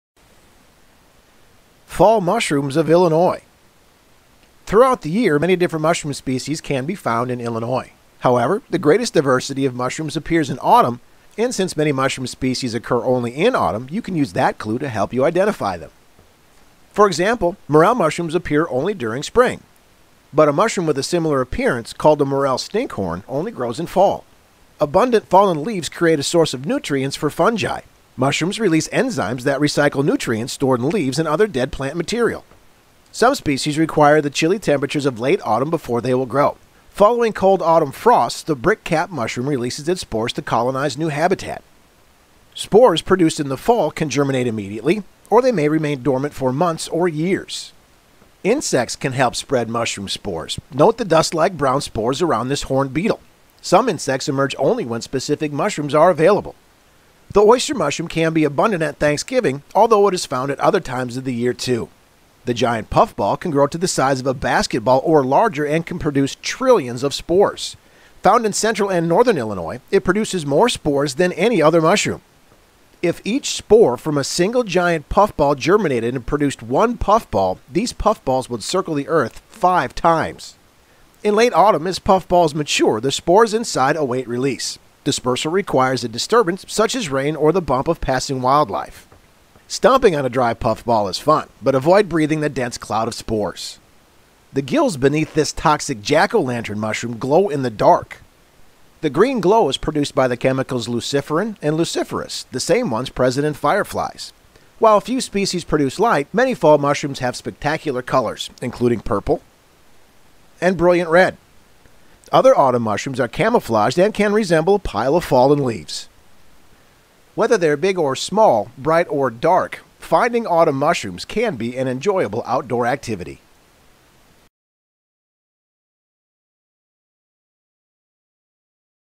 Fall Mushroom of Illinois
Terrific podcast of information regarding Fall Mushrooms of Illinois.